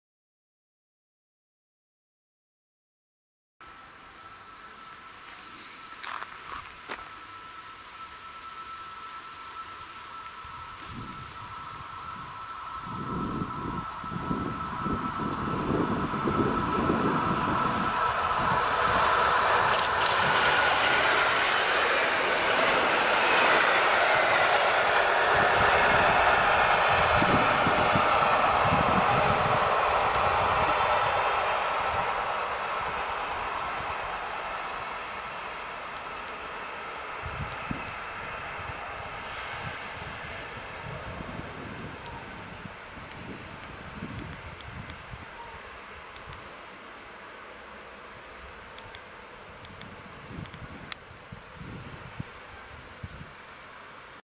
Wakacyjny jamniko-kibel
EN71-017 z R 50471 relacji Bydgoszcz Główna-Gdynia Główna zbliża się do p.o. Różyny.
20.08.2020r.